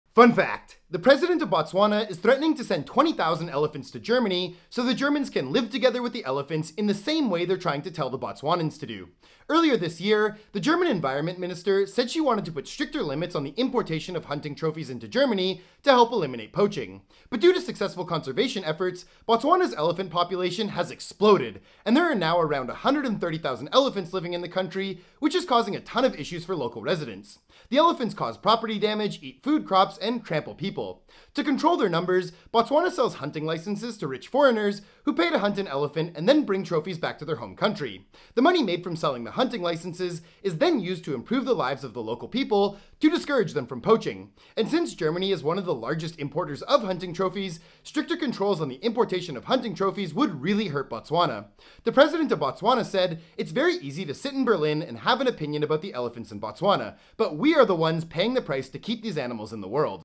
Botswana Threatens To Send 20,000 Elephants To Germany
The president of Botswana is threatening to send 20,000 elephants to Germany, so the Germans can live together with the elephants in the same way they’re trying to tell the Botswanans to do